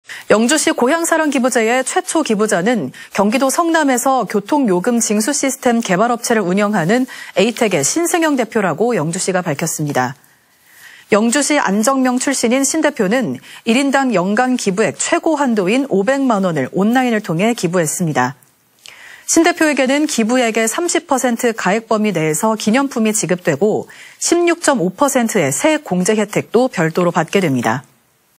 영주시, 고향사랑기부 첫 500만 원 '신승영 대표' / 안동MBC
2023/01/03 11:00:52    작성자 : 보도팀

영주시 고향사랑기부제의 최초 기부자는
경기도 성남에서 교통요금 징수 시스템
개발 업체를 운영하는 (주)에이텍의
신승영 대표라고 영주시가 밝혔습니다.
◀ＥＮＤ▶

영주시 안정면 출신인 신 대표는
1인당 연간 기부액 최고 한도인 500만 원을
온라인을 통해 기부했습니다.

신 대표에게는 기부액의 30% 가액 범위 내에서
기념품이 지급되고 16.5%의 세액 공제 혜택도
별도로 받게 됩니다.